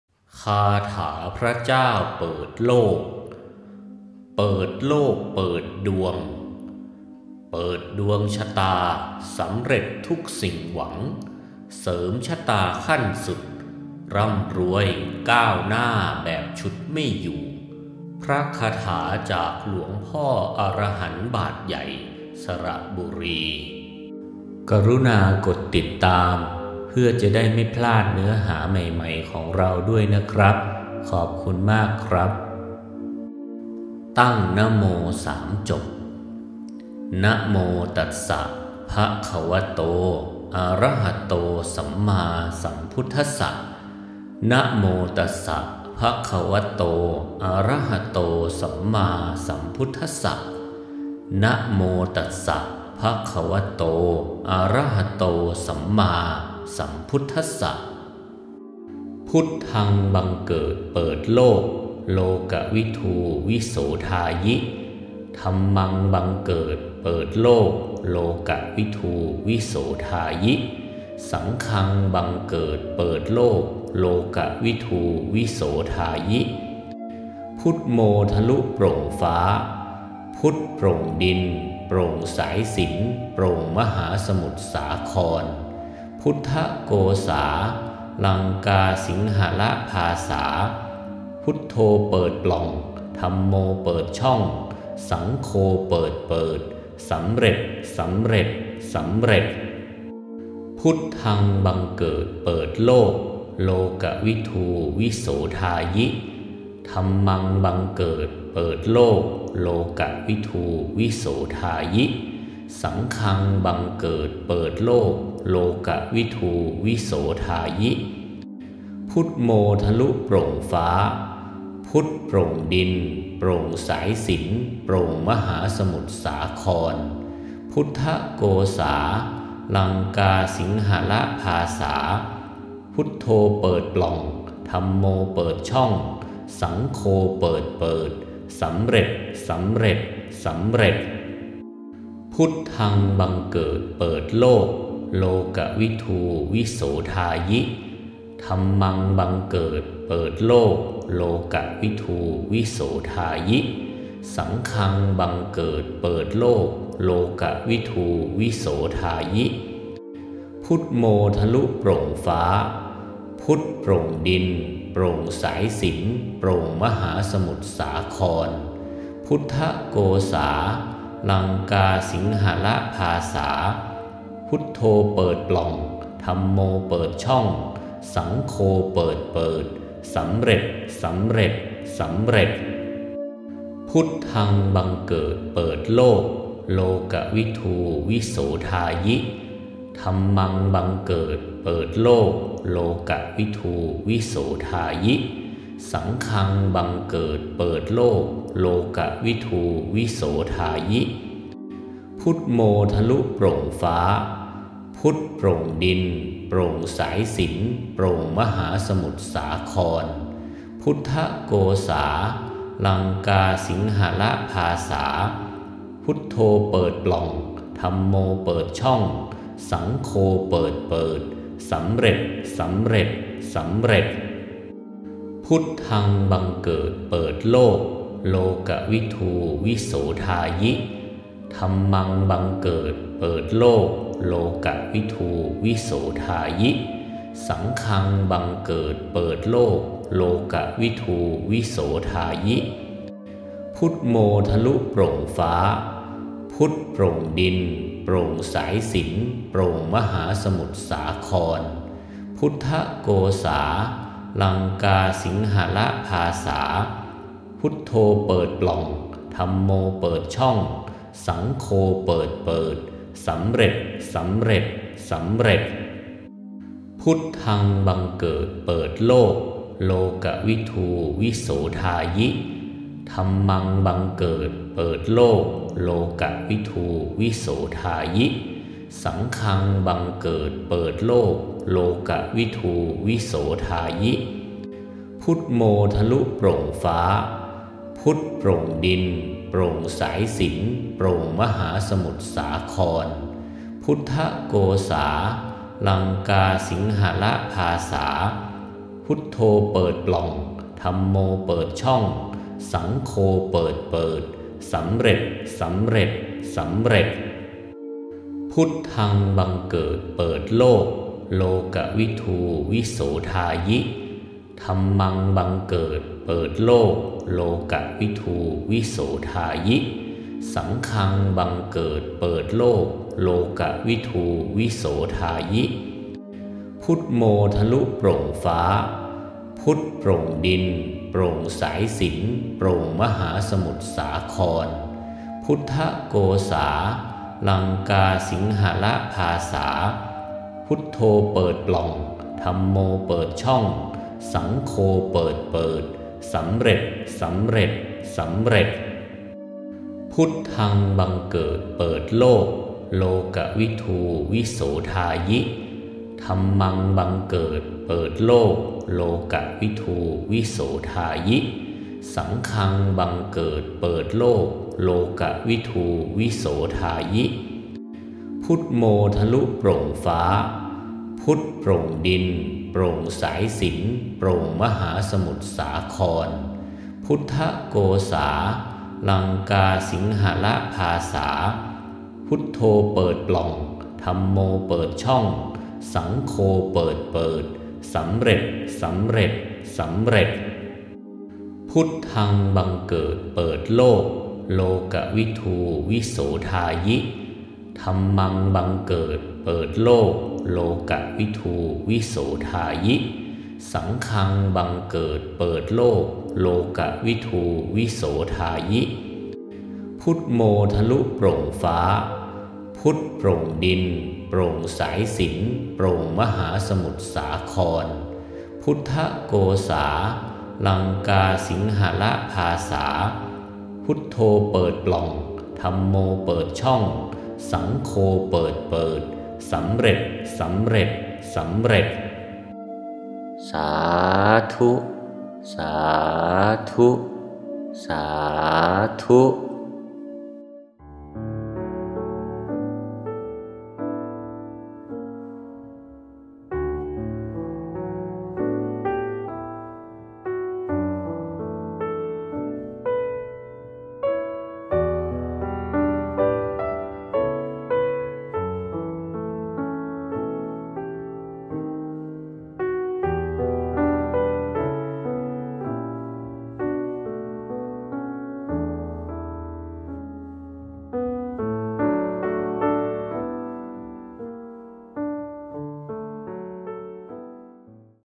คาถาพระเจ้าเปิดโลก เปิดโลกเปิดดวง เปิดดวงชะตา สมหวังทุกสิ่ง
คาถาพระเจ้าเปิดโลก เป็นพระคาถาของหลวงพ่อ อะระหันต์บาตรใหญ่ จ.สระบุรี
พระคาถาบทนนี้นิยมสวดภาวนา ทำสมาธิ เพื่อให้เกิดเสน่ห์ เมตตามหานิยมแก่ผู้สวด
เปิดดวงชะตาให้ลุกโชติช่วงชัชวาล ปัดทุกข์ปัดโศก ปัดเคราะห์กรรมที่ติดตามมา
ปัดสิ่งที่เป็นอุปสรรคต่างๆ ของชีวิต ช่วยให้ชีวิตดีขึ้นทันตาเห็น
หากสวดภาวนาเป็นประจำ หรือเปิดฟังก่อนนอน จะช่วยเสริมดวง เสริมโชคลาภ
คิดสิ่งใด กระทำการสิ่งใดก็จะสมมาตรปรารถนา ไร้ปัญหาและอุปสรรค

มาเริ่มสวดกันเลยนะครับ
เริ่มจากตั้งนะโม 3 จบ
แล้วเริ่มสวดคาถาพระเจ้าเปิดโลก (9 จบ)

พุทธัง บังเกิด เปิดโลก โลกะวิทู วิโสธายิ
ธัมมังบังเกิด เปิดโลก โลกะวิทู วิโสธายิ
สังฆังบังเกิด เปิดโลก โลกะวิทู วิโสธายิ
พุทโมทะลุโปร่งฟ้า พุทโปร่งดิน โปร่งสายสิญจ์ โปร่งมหาสมุทรสาคร

พุทธะโกษา ลังกาสิงหละภาษา พุทโธเปิดปล่อง ธัมโมเปิดช่อง สังโฆเปิดเปิด

สําเร็จ สําเร็จ สําเร็จ

เมื่อกล่าวบทสวดคาถาเสร็จสิ้น
ให้อธิฐานจิต ระลึกถึงคุณพระรัตนตรัย

และสิ่งศักดิ์สิทธิ์ทั้งหลาย เทพเทวดาทั้งมวล
จงช่วยดลบันดาลให้
ดวงชะตาจงเปิดไปสู่สิ่งดี
ให้สมหวังดังใจทุกประการ มีโชคมีลาภ
ให้ชีวิตเปลี่ยนไปสู่สิ่งที่ดี ประสบความสำเร็จ
ในทุกสิ่งที่ทำ
ให้ชีวิตเจริญก้าวหน้า
ราบรื่นรุ่งเรือง ปราศจากปัญหาและอุปสรรค
ด้วยเทอญ..

"จิตยิ่งบริสุทธิ์ คาถายิ่งสำฤทธิ์ผล"
ขอให้ท่านทั้งหลาย ประพฤติ ปฏิบัติตัวให้อยู่ในศีล ในธรรม
หมั่นทำบุญ ทำทาน สร้างกุศล จะช่วยให้โชคดี มีสุข ร่ำรวยๆ
คิดหวังสิ่งใด ก็จะสมปรารถนาทุกประการ..

บุญรักษาครับทุกท่าน.